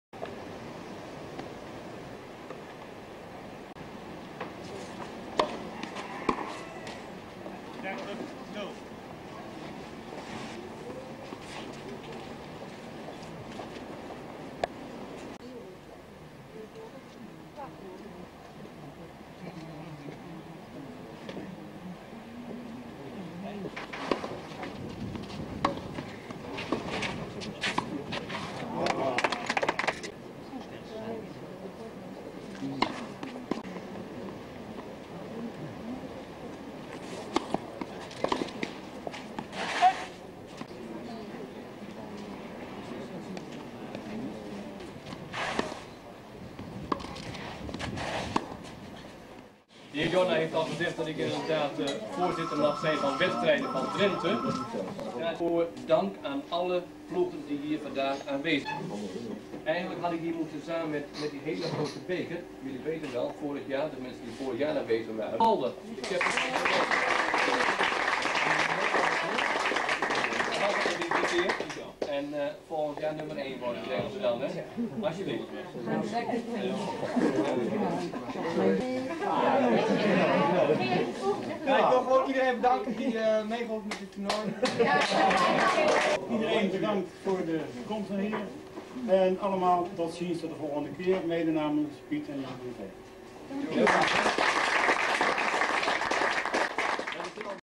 TCSuthwalda50 1992 NJKenDS /video robertmeyer
Publicatie in kader 50 jarig jubileum TC Suthwalda (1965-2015): Nederlandse Jeugdtennis-Kampioenschappen en Dorpen-en-Steden-toernooi Drenthe in 1992.